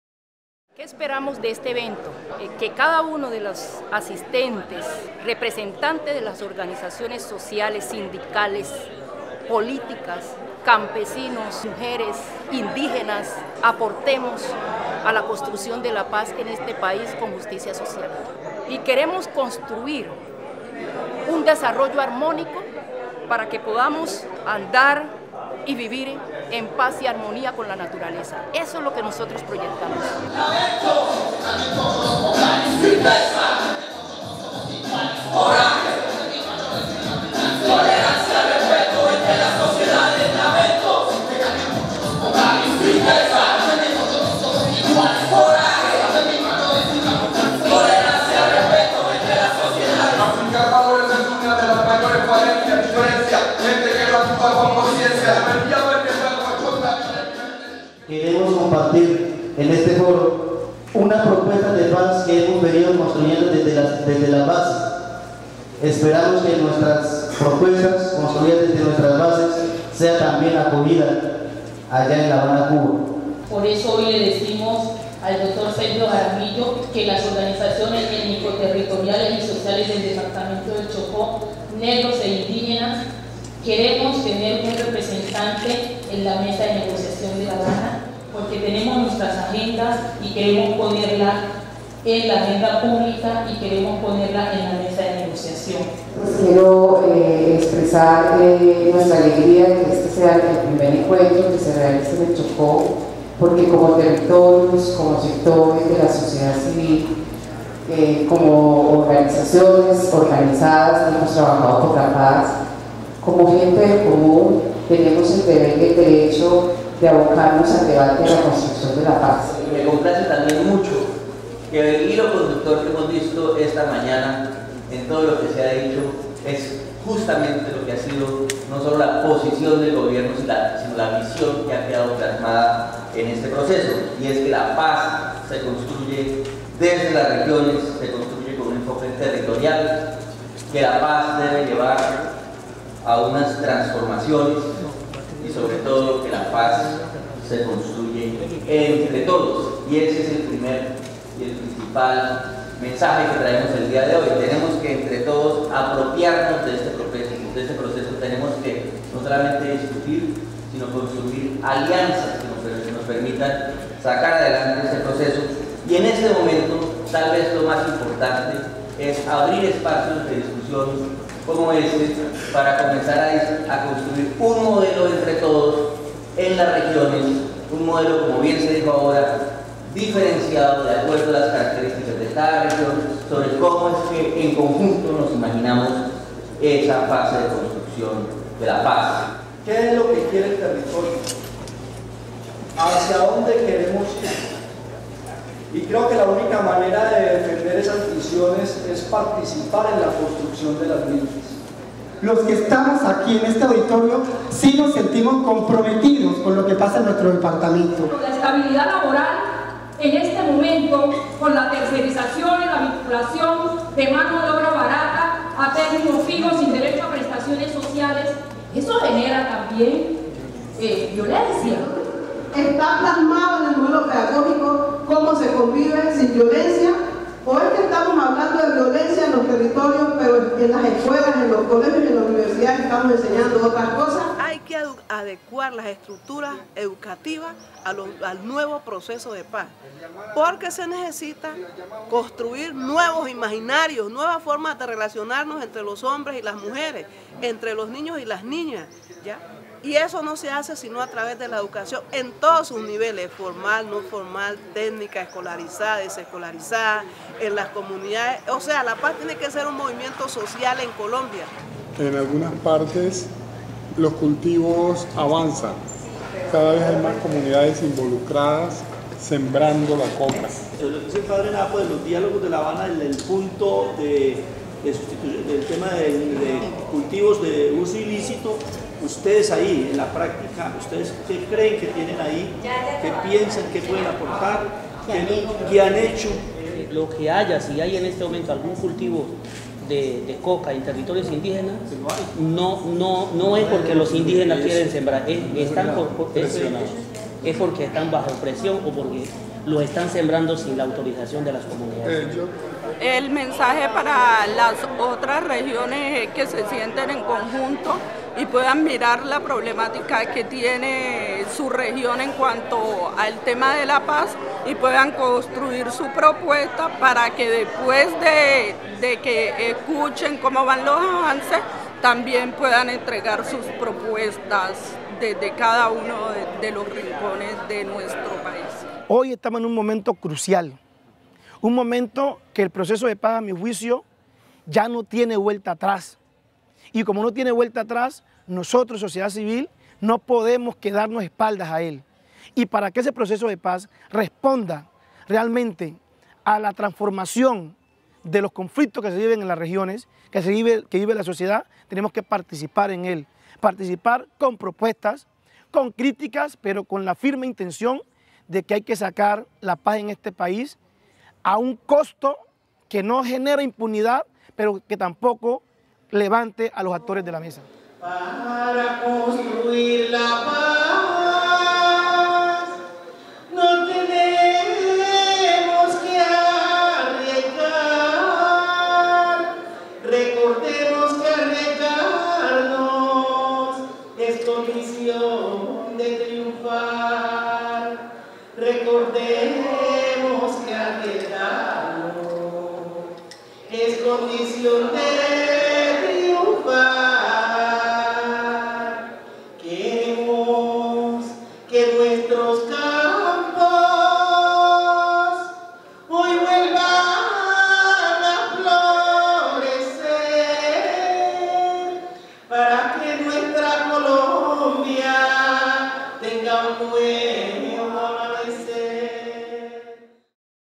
ENCUENTRO REGIONAL DE PAZ - QUIBDÓ
El 31 de Julio de 2014 se realizó en Quibdó (Chocó) el Primer encuentro Regional para la Paz. Estos encuentros se llevarán a cabo en 12 regiones del país y tienen como objetivo movilizar a la sociedad civil hacia la construcción de la paz desde los territorios.